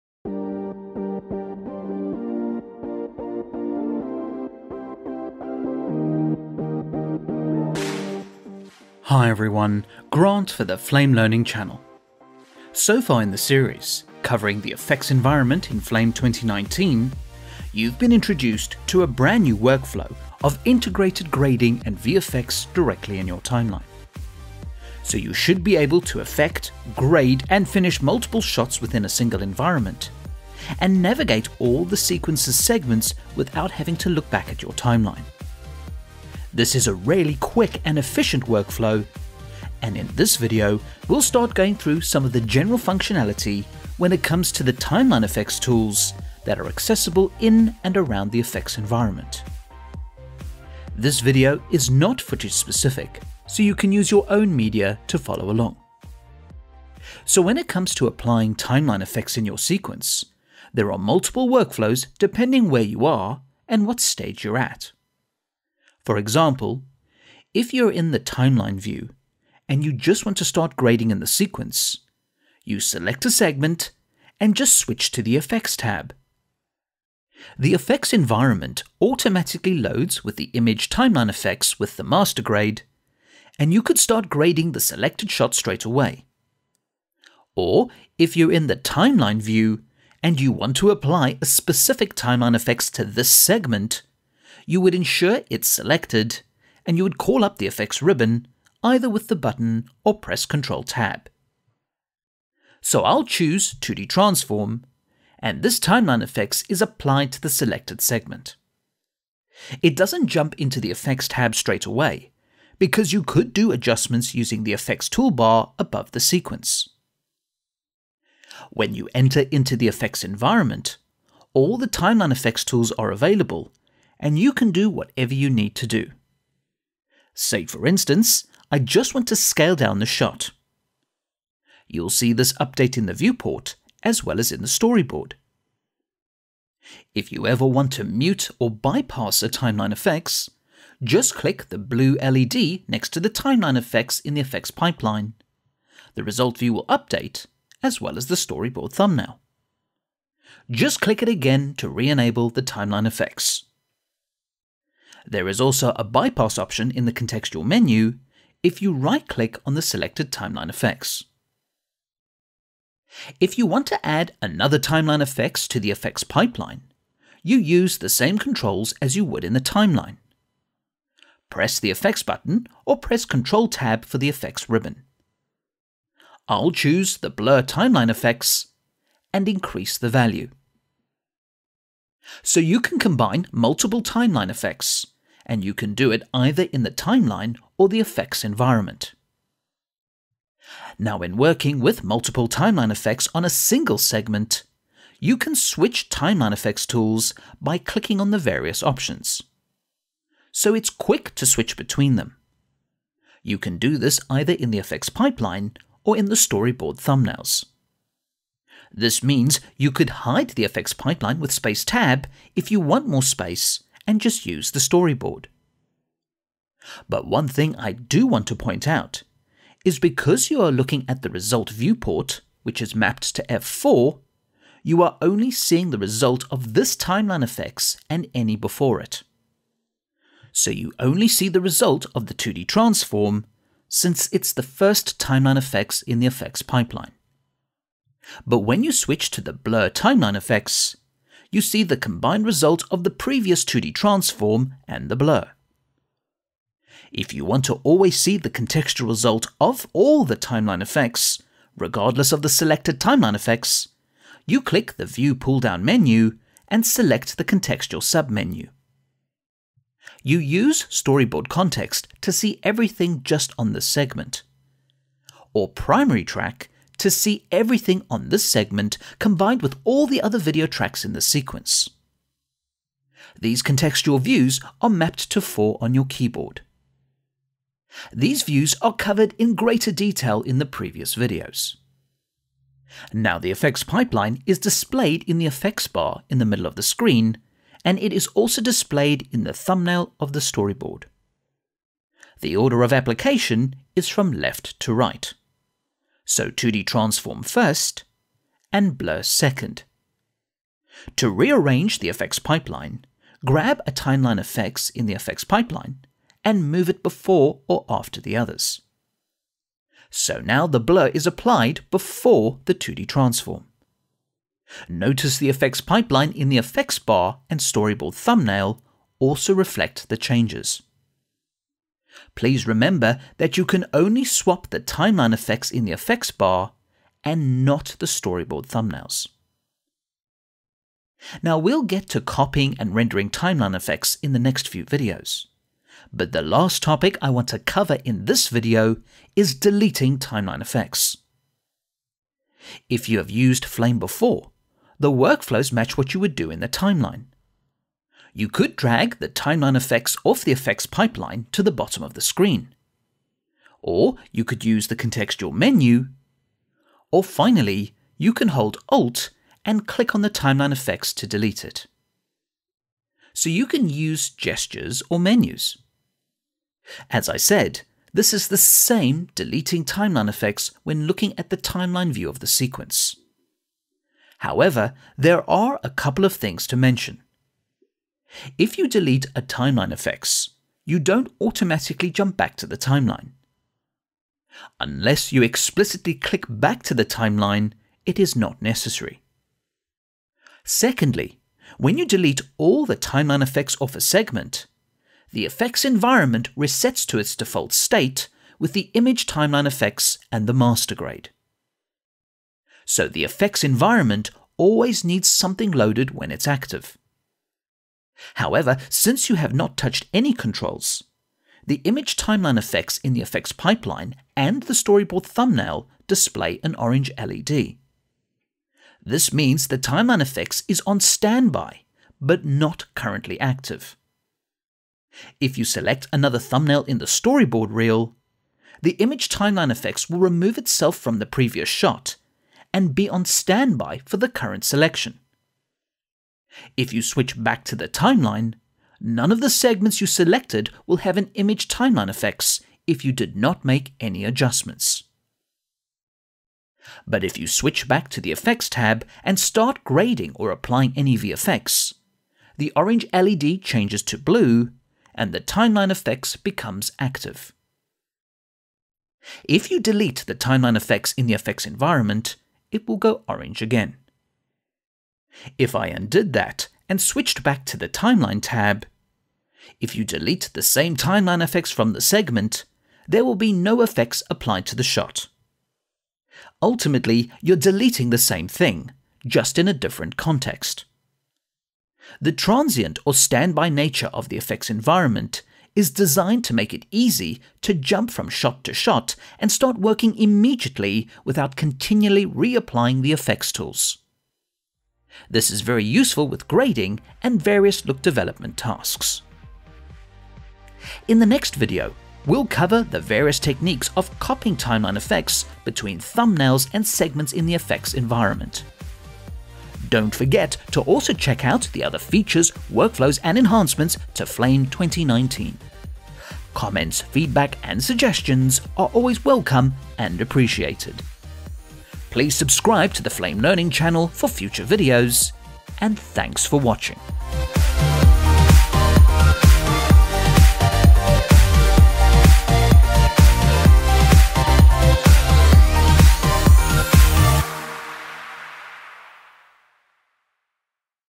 Effects Environment – Part 8 - Working with TimelineFX - Flame 2019
Level: Beginner

In this video, you’ll start going through some of the general functionality when it comes to the TimelineFX tools that are accessible in and around the EFFECTS environment.

This covers TimelineFX indications, the FX pipeline, working with single or multiple TimelineFX, interacting with the FX-Pipeline or Storyboard Reel and deleting TimelineFX.

If you wish to follow along, you can download the 30-day Flame trial